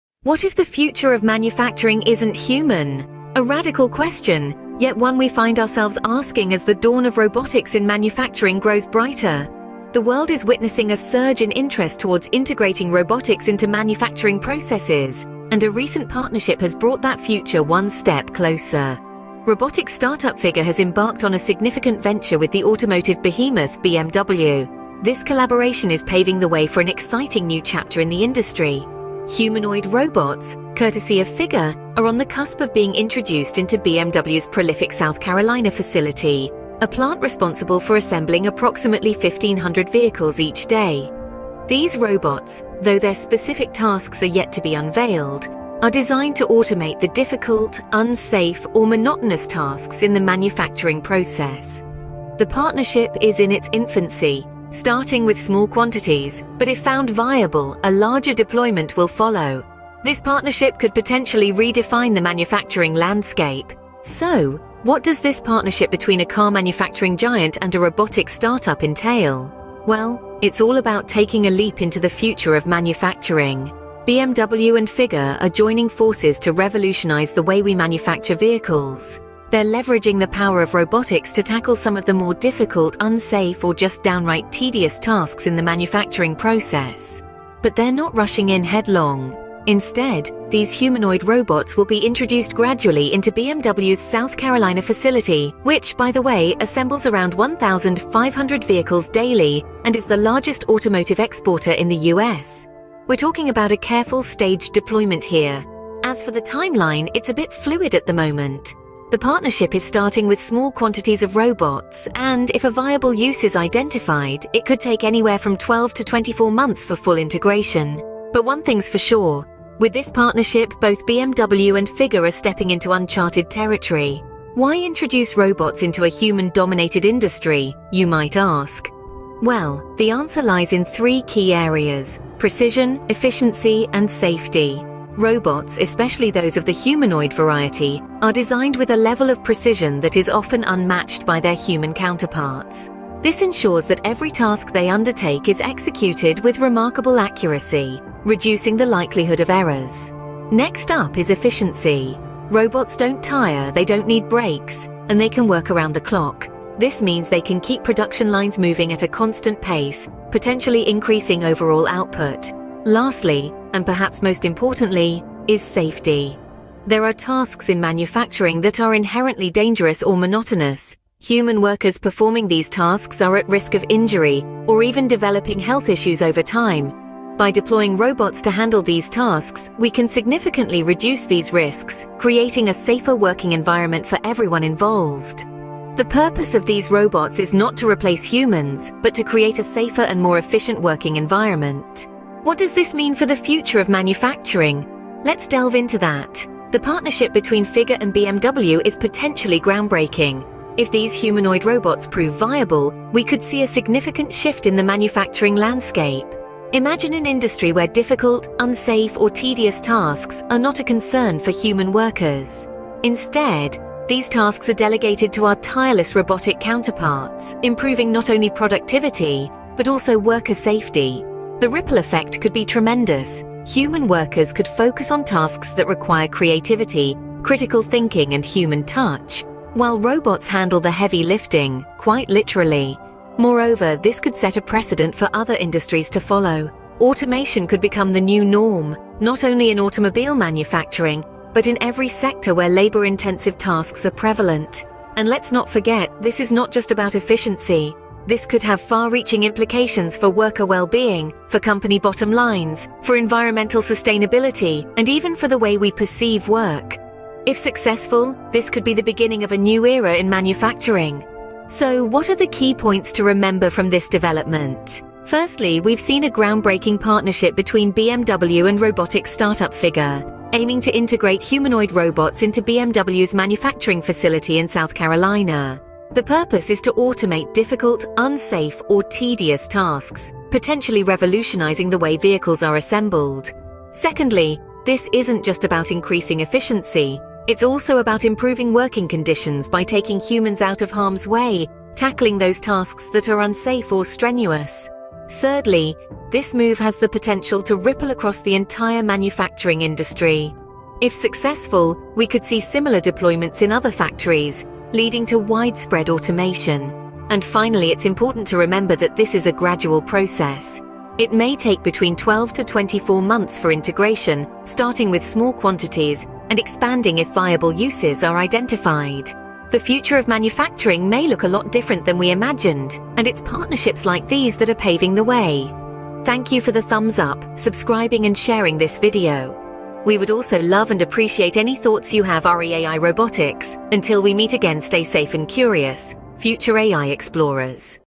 The Rise of Robotics in Manufacturing: A Game-Changing Partnership
Welcome to FuturAi Explorers,
In an exciting development, BMW is revolutionizing its operations at the South Carolina facility by introducing humanoid robots. These cutting-edge machines are set to transform the automotive industry with their unprecedented capabilities. In this video by The Verge, we dive into BMW's groundbreaking move and unravel the potential impact it could have on the future of production. Join us as we explore how these humanoid robots are being integrated into day-to-day activities and what advantages they bring to the table. Prepare to be amazed by the seamless merging of human-like abilities with advanced technological prowess. Stay up-to-date with the latest innovation in the automotive world and witness the advent of a new era at BMW's South Carolina facility. Watch now to get an exclusive glimpse into this remarkable feat!